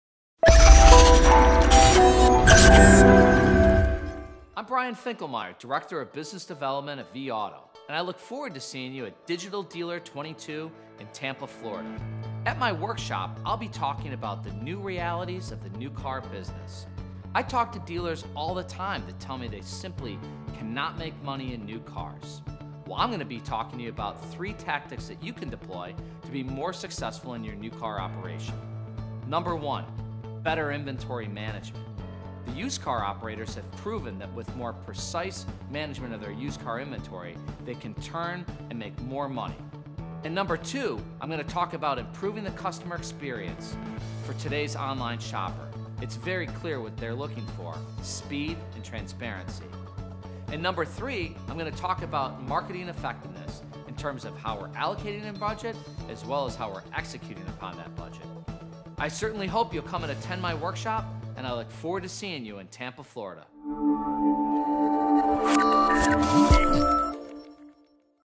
Brian Finkelmeyer - New Realities of the New Car Business
Dealers are under enormous pressure in today's new car market - near record levels of inventory, pricing transparency websites, and factory volume bonus programs all contribute to razor thin new car margins. Dealers must respond to these challenges by improving their operational efficiency in three key areas: inventory management, customer engagement, and marketing. In recent years, dealers have increasingly leaned on fixed operations and F&I to maintain profitability as new and used margins continue to slide. Most industry analysts believe the U.S. new car market has plateaued, which is going to create even more pressure on dealers. The time is now for reexamining new car operations to improve efficiency and build profitable operations for the future.